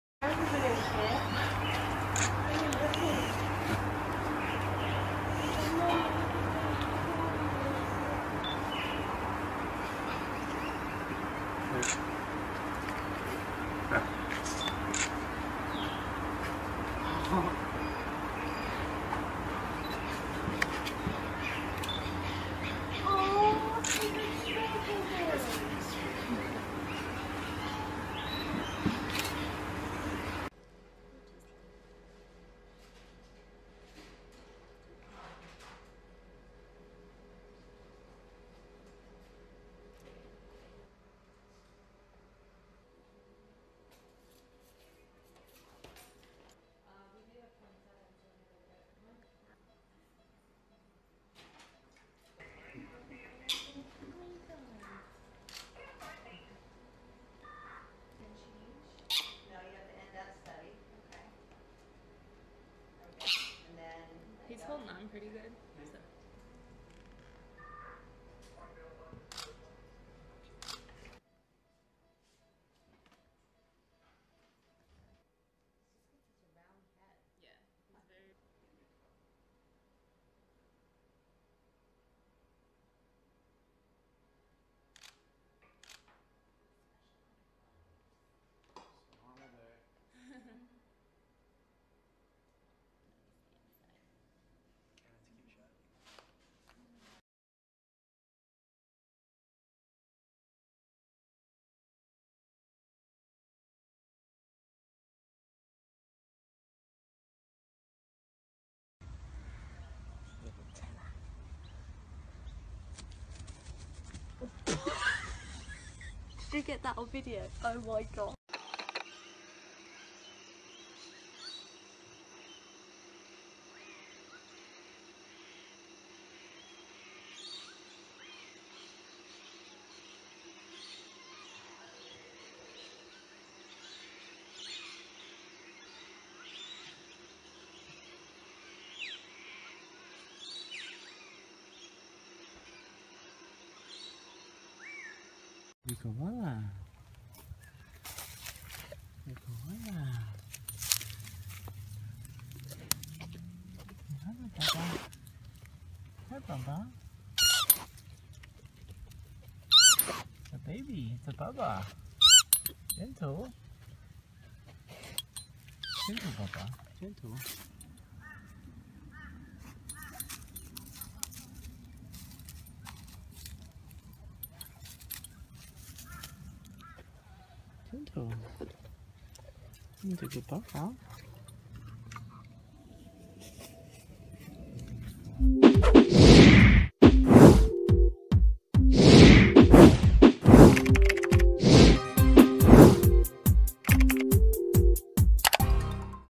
KOALA - A Cute Koala Videos And Funny Koala Bear Compilation || PETS And ANIMALS (2021)
Check out these cute koala videos and funny koala videos in this koalas bear compilation. Koala bears are very cute and funny animals and pets.